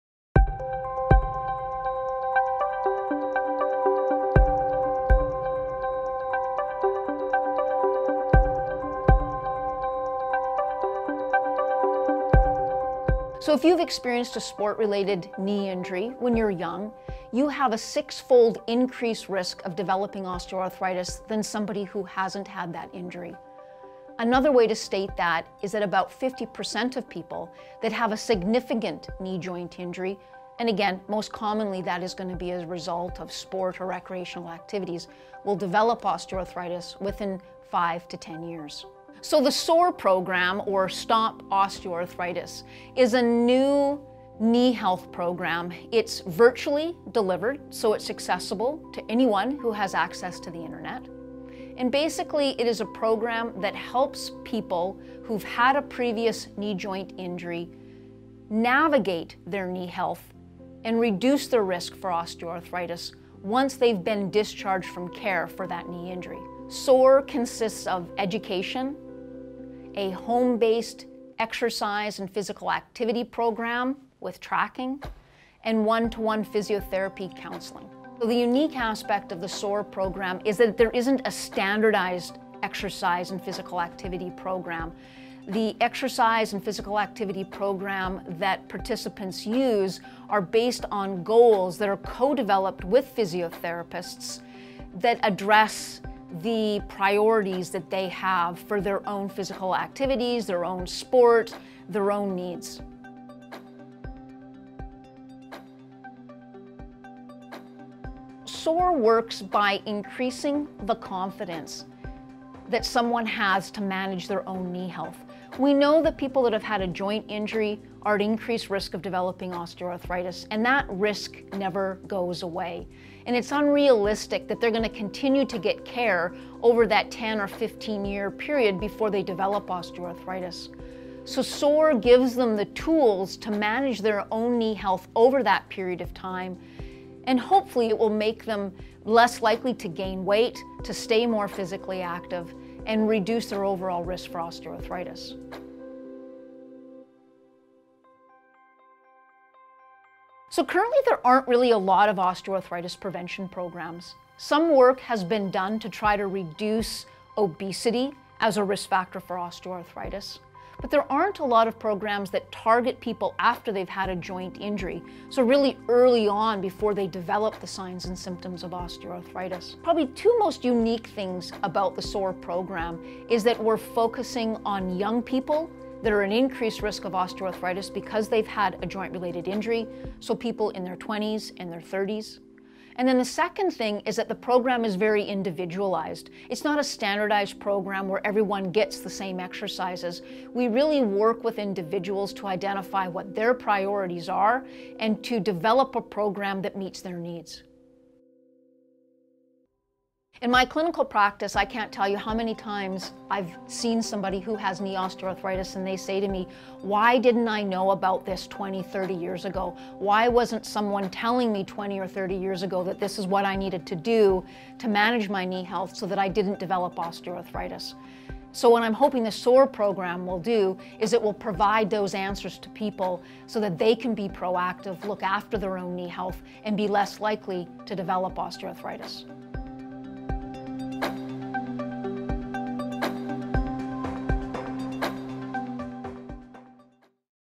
Osteoarthritis Research
About 50% of people who experience a knee joint injury will develop osteoarthritis within five to 10 years. This is a serious problem.

We've created a unique program that aims to help prevent osteoarthritis after a sport-related knee injury. SOAR, or STOP OsteoARthritis, is a new knee health program. It’s virtually delivered and designed to help people, who have had a previous knee joint injury, navigate their knee health and reduce their risk for osteoarthritis once they’ve been discharged from care for that knee injury.

Learn more from the research scientist behind the program, Dr. Jackie Whittaker.